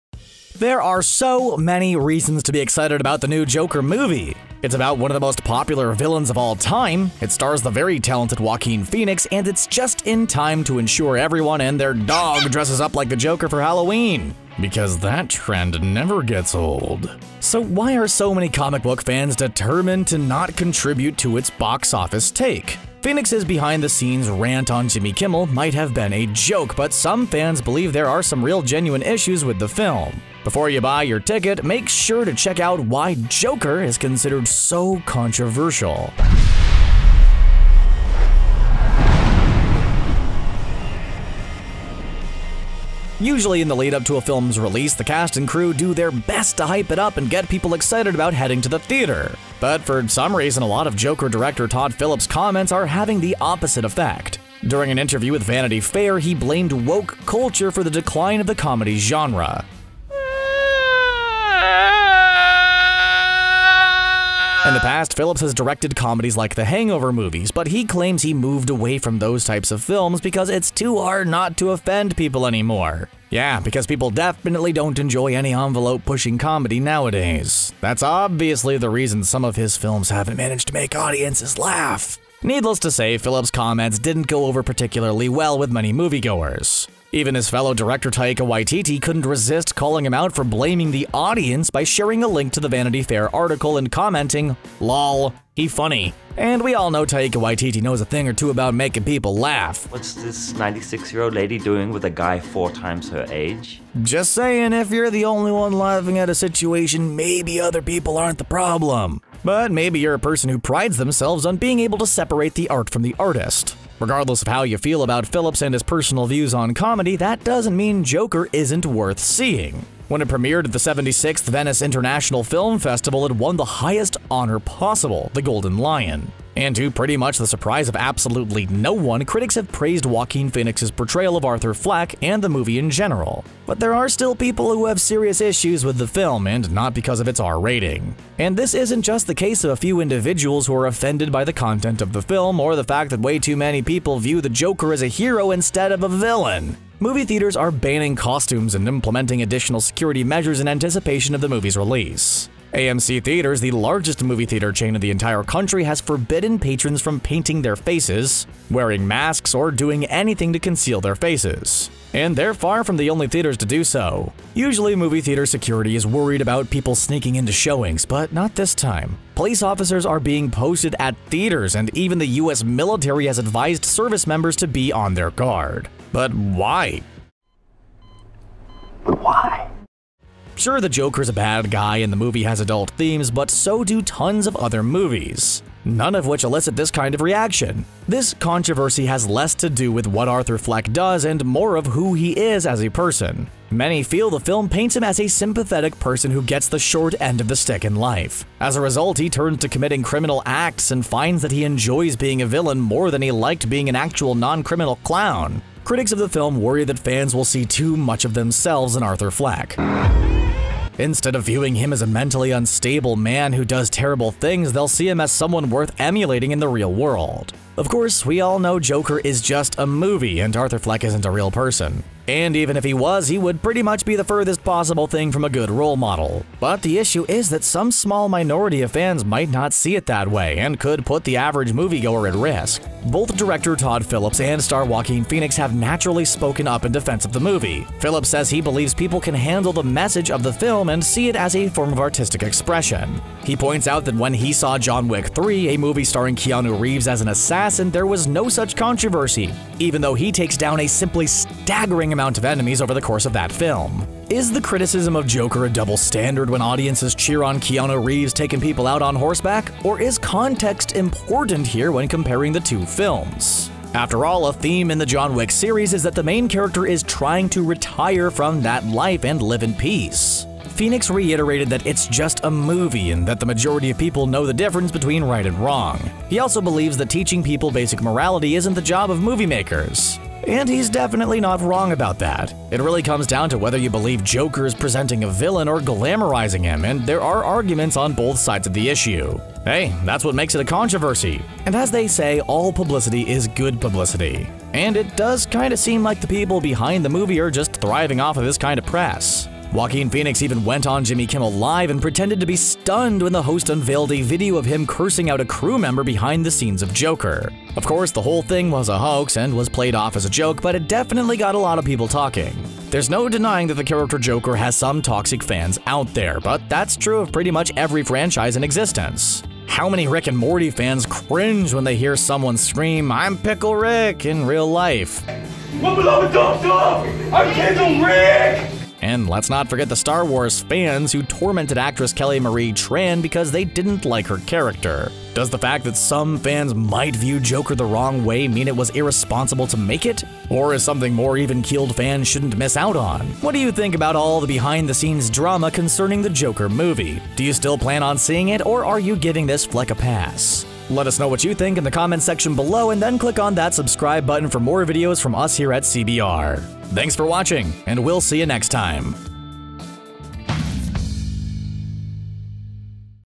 Joker Is The Opposite Of Woke Culture
Why The New Joker Movie Is Pretty Controversial

If you love DC Comics then you’re probably excited about the fact that we get a brand new Joker movie to wash the taste of Suicide Squad out of our mouths. But the latest flick might be more controversial than you might think. Director Todd Phillips made some statements about why he abandoned the comedy genre that some fans had issues with. Even Thor: Ragnarok director Taika Waititi seemed to think there might be other reasons people weren’t laughing at the Hangover movies. We’ll explain why some movie theaters are taking additional precautions and banning face paint as they get ready to show the film. Joaquin Phoenix portrays Arthur Fleck and he has some strong opinions on why the film is worth seeing despite all of the issues some people have with it. Come on, you weren’t fooled by his behind the scenes outburst on Jimmy Kimmel Live! Were you? They say all publicity is good publicity and if that’s true, this flick might just break some serious records.

What do you think about all of the drama surrounding the latest movie from Warner Bros. studio? Are you going to head to your local cinema regardless or are you thinking about waiting until it comes out on video and you can watch it in the safety of your own home?